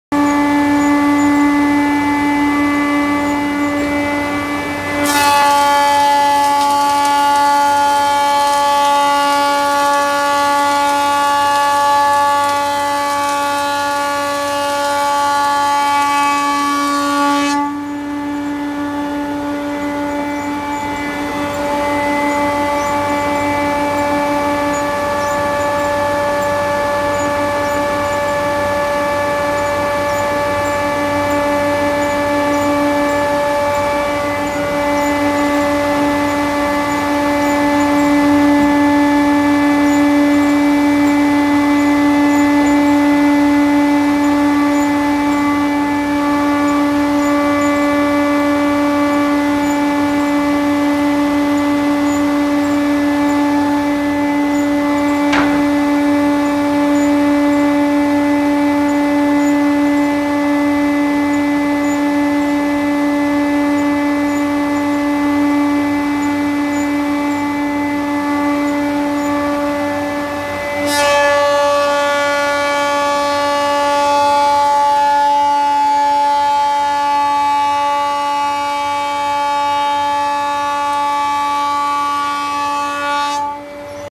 Plaining the samson post - Mayan
Taking the raw purple heart beam down to the size for the new samson post